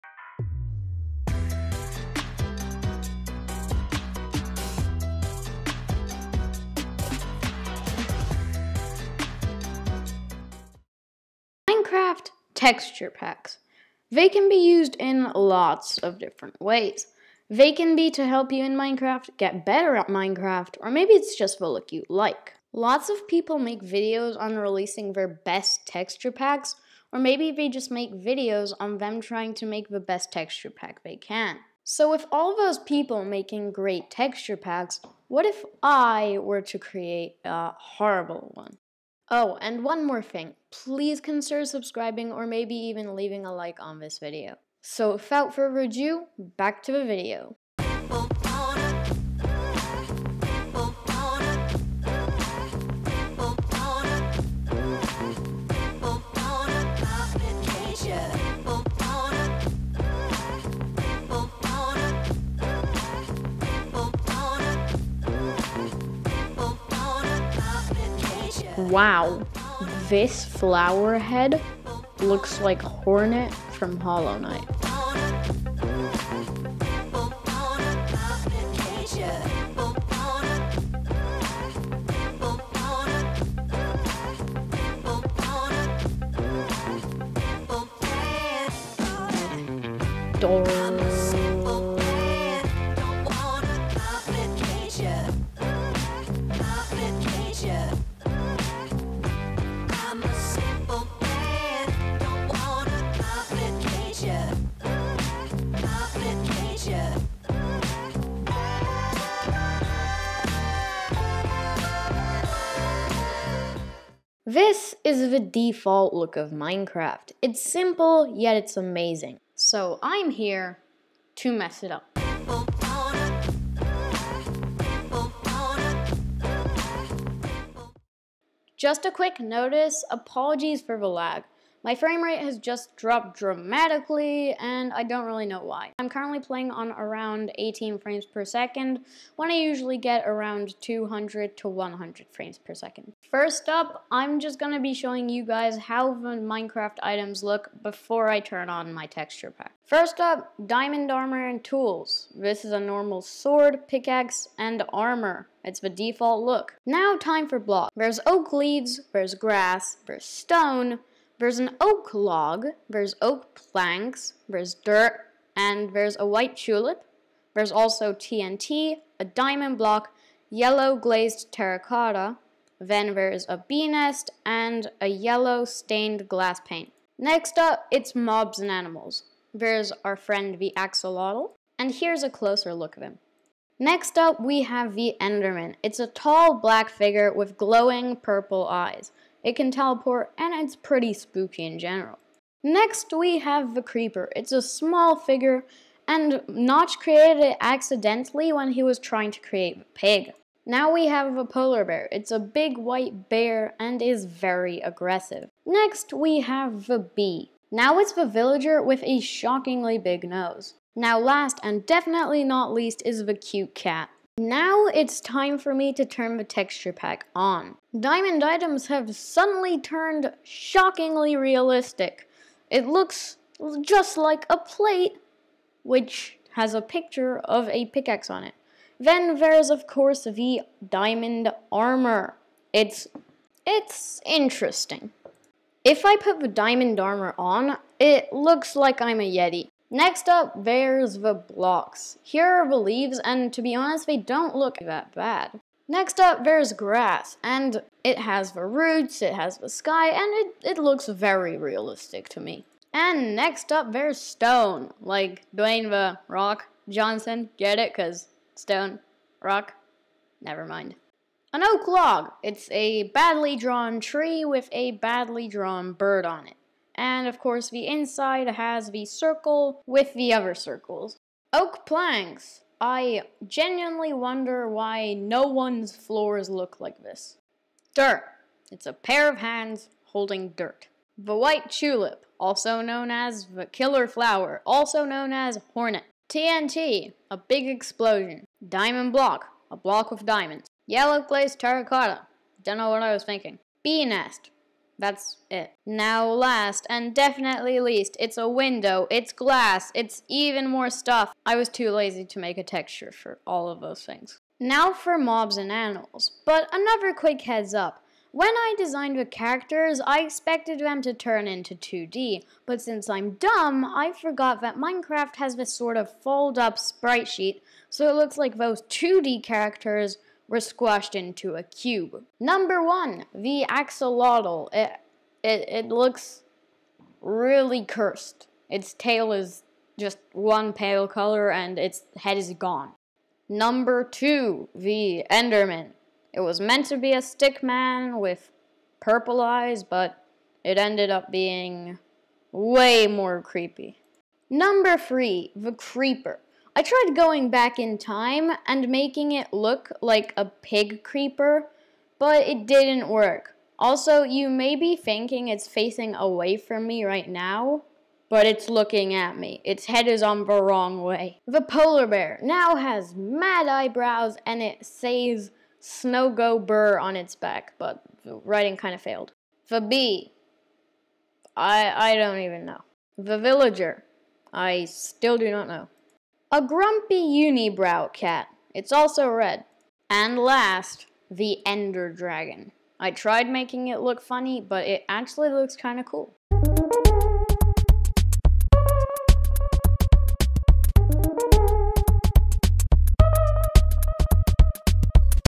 I made the worlds WORST texture pack...
Hello! In this video, I will be showing you guys a texture pack that I made. Feel free to vomit.